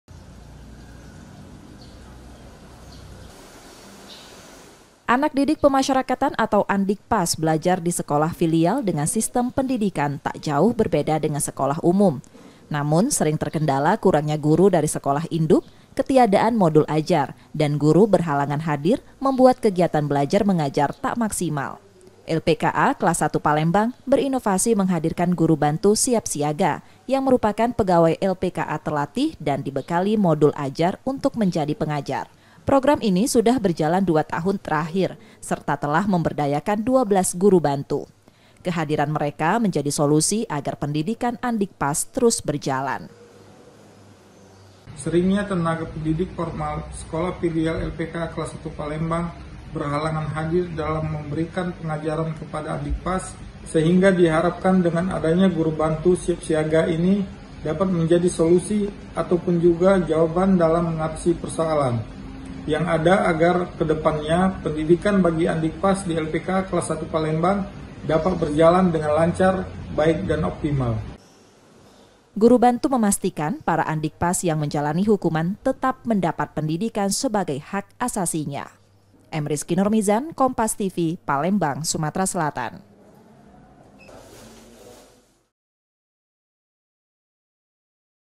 Guru Bantu Siap Siaga Sekolah Filial di LPKA Palembang
PALEMBANG, KOMPAS.TV - Kegiatan belajar mengajar di sekolah filial lpka palembang, sering terkendala guru yang tak hadir. Hal tersebut mendorong lpka berinovasi, menghadirkan guru bantu siap siaga.

Anak didik pemasyarakatan atau andikpas belajar di sekolah filial, dengan sistem pendidikan tak jauh berbeda dengan sekolah umum. Namun sering terkendala kurangnya guru dari sekolah induk, ketiadaan modul ajar, dan guru berhalangan hadir, membuat kegiatan belajar mengajar tak maksimal.

LPKA kelas i palembang, berinovasi menghadirkan guru bantu siap siaga, yang merupakan pegawai lpka terlatih dan dibekali modul ajar, untuk menjadi pengajar.

Program ini sudah berjalan dua tahun terakhir, serta telah memberdayakan 12 guru bantu. Kehadiran mereka menjadi solusi/ agar pendidikan andikpas terus berjalan.

Guru bantu memastikan para andikpas yang menjalani hukuman, tetap mendapat pendidikan sebagai hak asasinya.